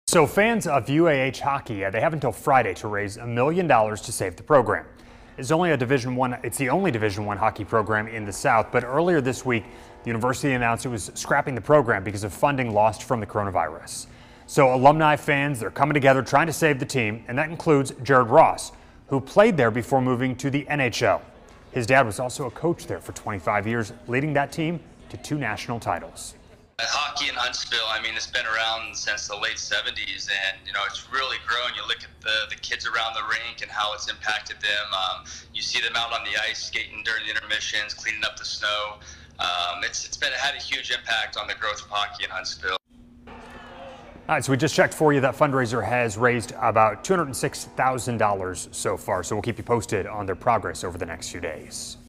Push to save UAH hockey program
A crowdfunding effort is underway to save the University of Alabama-Huntsville hockey program. Learn more in the video above.